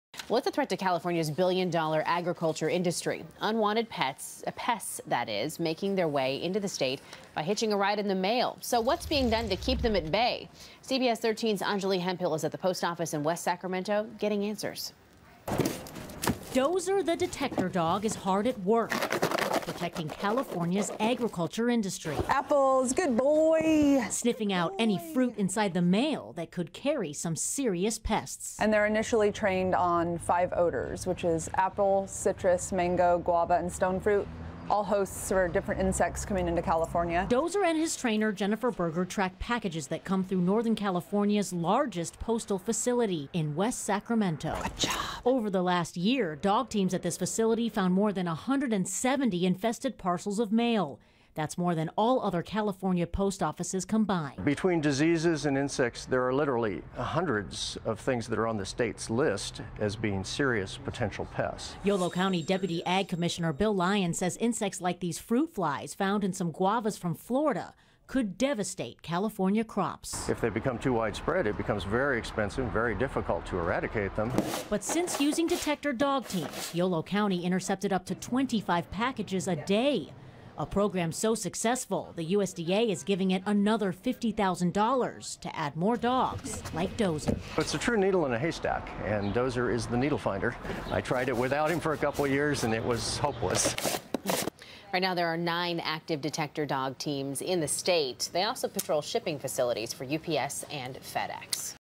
Dozer The Detector Dog Helps Keep Mail Pests Away From Yolo County Agriculture
&nbsp;Over the last year, dog teams at the facility found more than 170 infested parcels of mail. That's more than all California post offices combined.